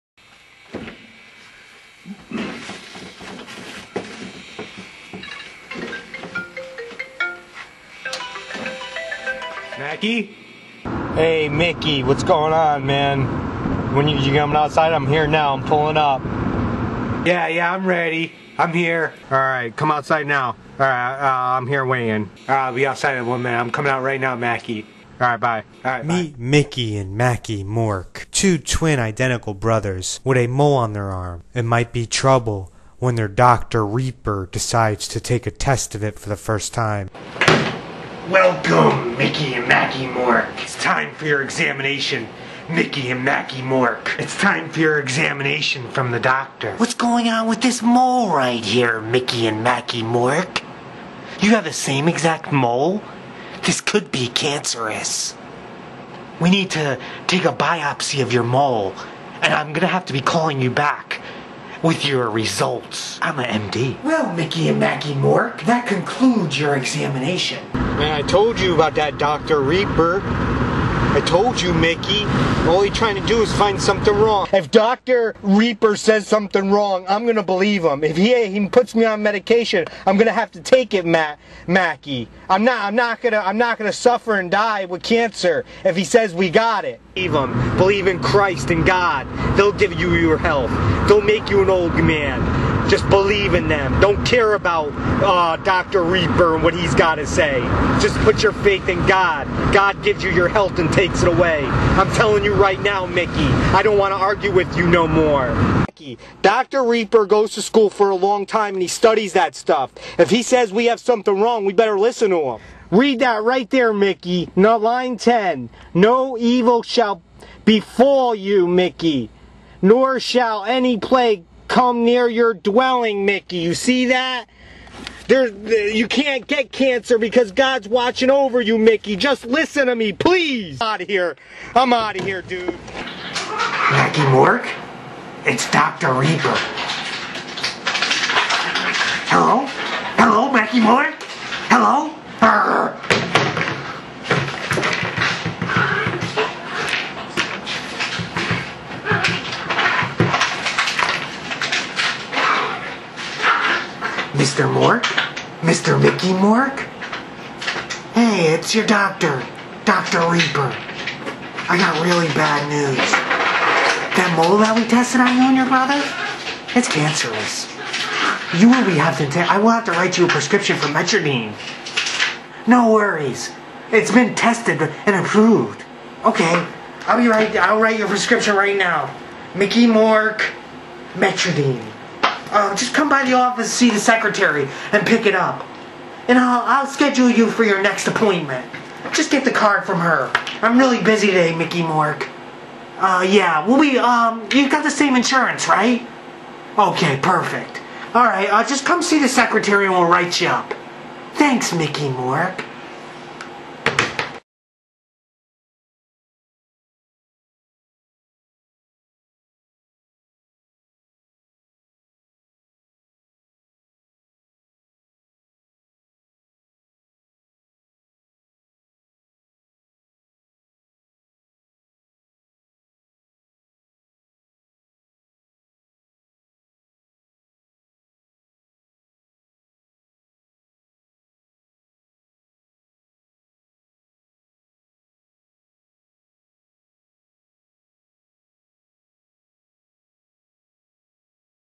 Doctor Visit Bad News Or Is It
I play all the characters in this film after I produced it I seen numerous articles about doctors prescribing unnecessary medication for a profit. Mickey and Mackey Mork Identical brothers go to doctor Reaper for an examination,  doctor Reaper gives bad news to the twins but one has faith in something else not doctor reaper but one brother believes in doctor Reaper what happens next watch an see.